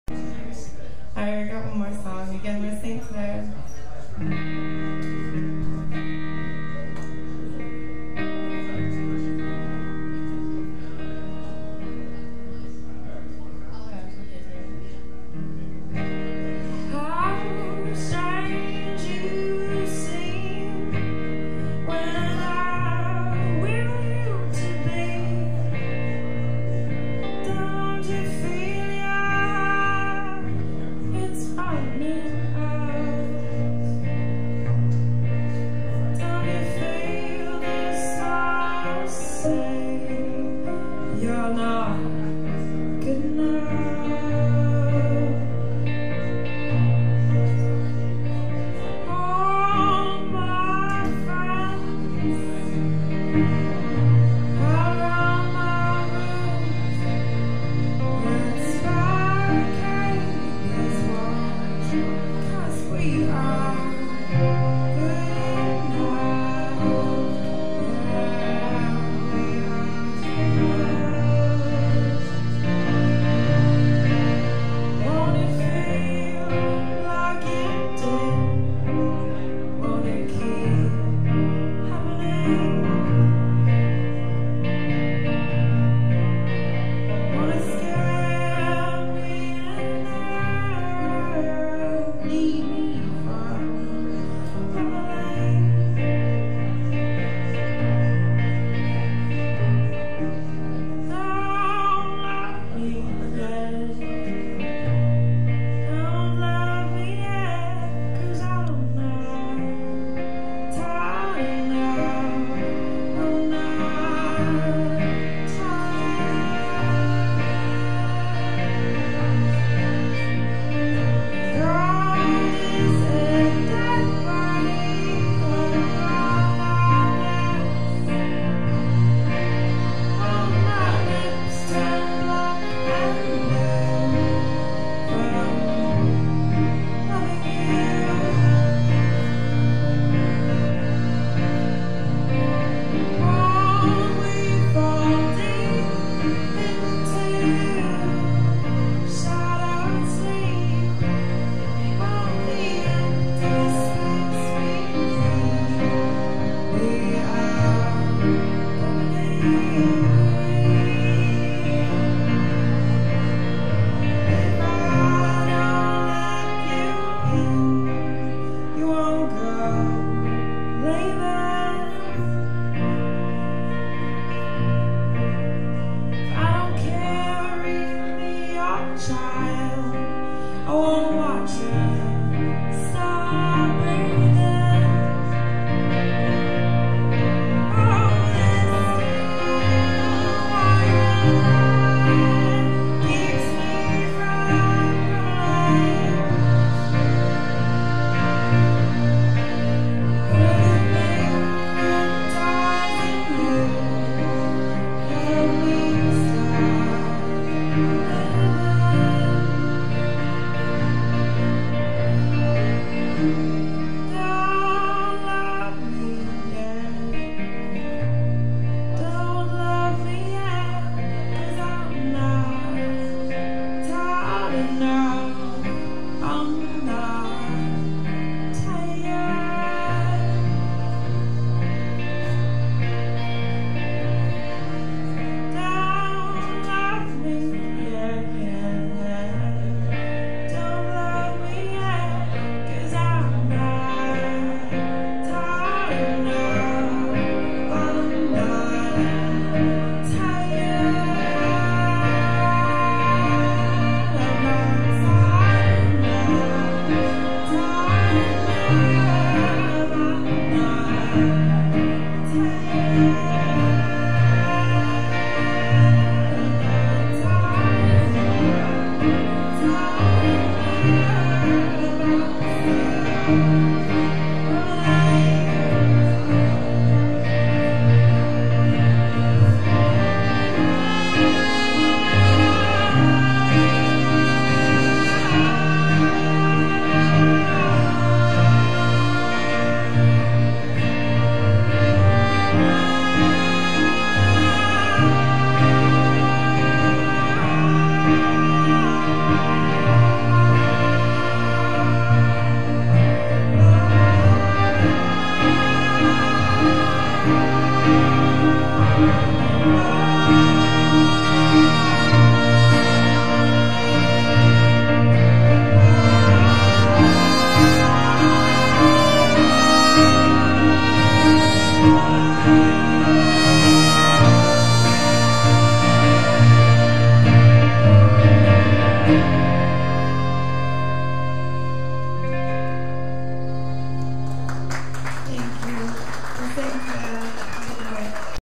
St. Claire at O'Brien's Pub, Allston MA 5/19/10
Another wistful, evocative tune by a wistful, evocative band with Brian Rutledge adding some crescendoing trumpet from the ghostly shadows on the left. Some serious contrast adjustments were needed here, resulting in the curious look of the video. Shot with a Zi8 (720p 60fps)/Sony ECM-DS70P mic combo.